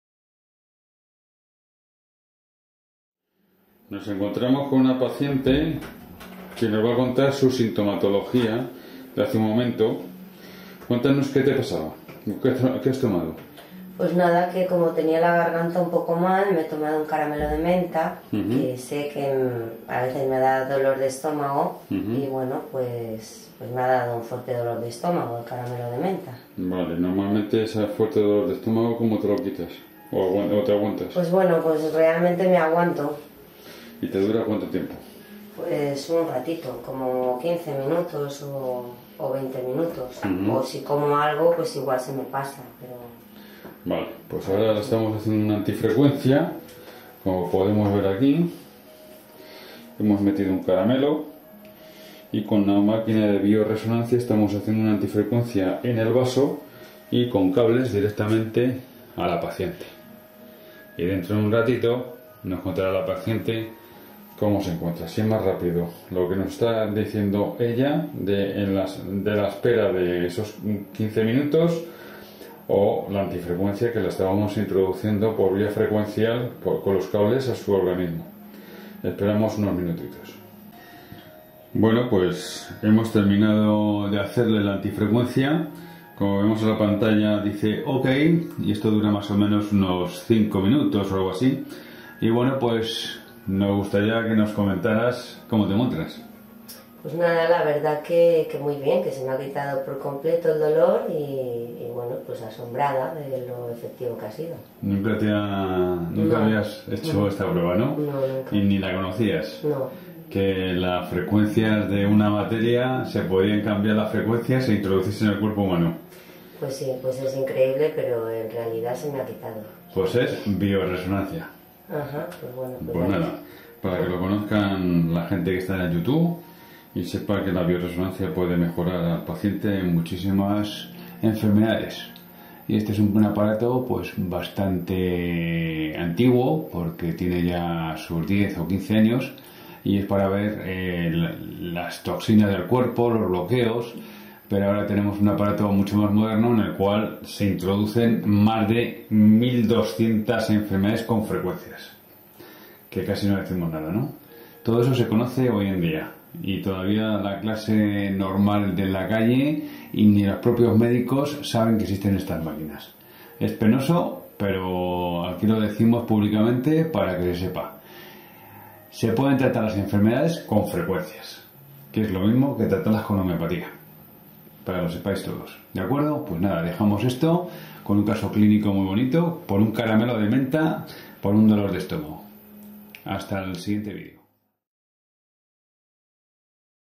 DOLOR DE ESTOMAGO POR UN CARAMELO DE MENTA
Paciente que acude a consulta y por tomar un caramelo de menta por picazón de garganta, comienza a dolerla el estómago, se lo quitamos con biorresonancia.